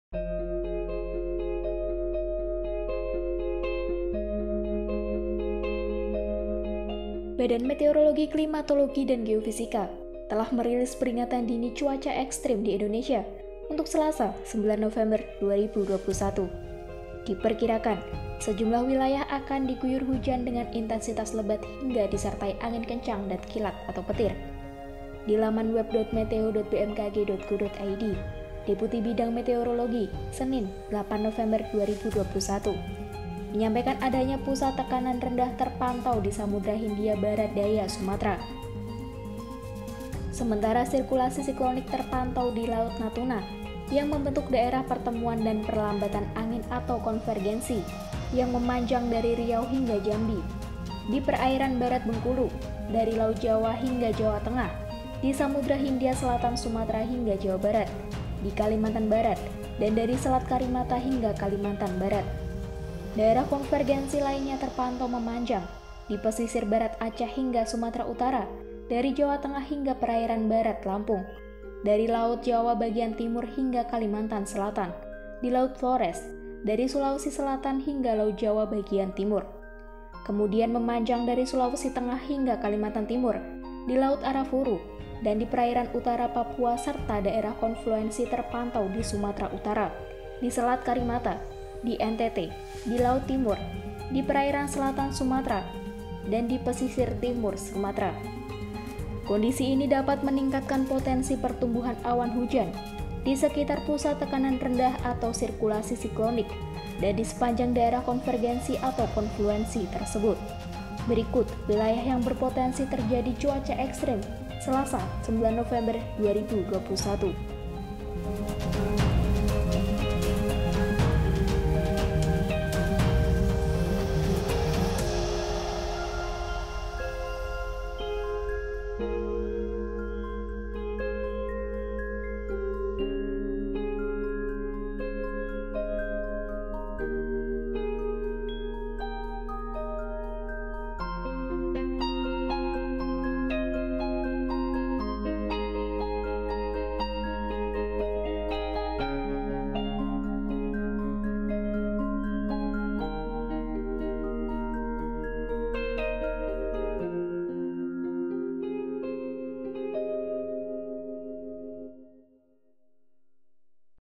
Prakiraan Cuaca BMKG 9 November 2021, 26 Wilayah Diguyur Hujan Lebat
Badan Meteorologi, Klimatologi, dan Geofisika (BMKG) telah merilis peringatan dini cuaca ekstrem di Indonesia untuk Selasa (9/11/2021).  Diperkirakan, sejumlah wilayah akan diguyur hujan dengan intensitas lebat hingga disertai angin kencang dan kilat/petir.  Di laman web.meteo.bmkg.go.id, Deputi Bidang Meteorologi, Senin (8/11/2021), menyampaikan adanya pusat tekanan rendah terpantau di Samudera Hindia Barat Daya Sumatera.

(jek)